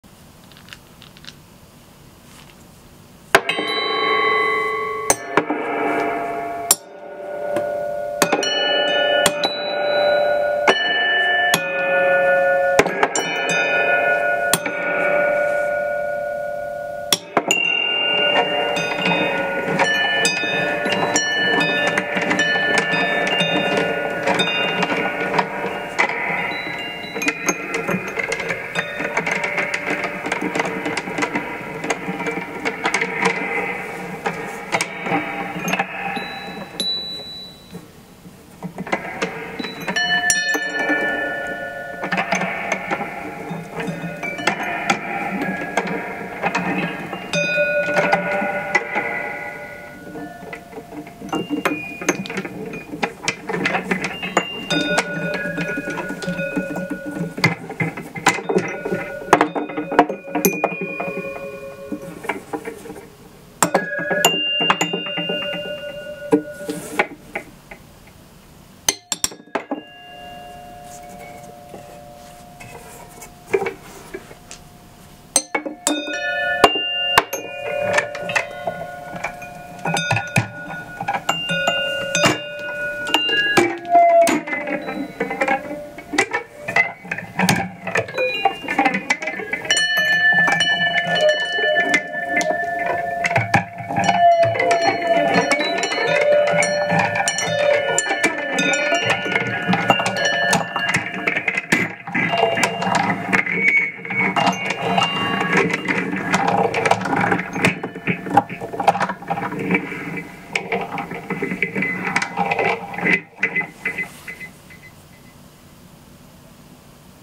New instrument
This instrument, constructed over the MLK weekend, incorporates various bells and metal tubes in a felt-lined wood frame. I'm still experimenting with pickups and strikers.
This example uses balls of cedar, but I've also tried other spherical objects, including metal Chinese massage balls, as well as mallets.
Everything in the building process -- except the casters, some nuts and bolts, and the cedar balls, all recently obtained at the neighborhood hardware store  -- was junk from the basement, collected over the years from yard sales, flea markets, medical facilities and my parents' house.
The instrument does not yet have a name. I welcome your suggestions.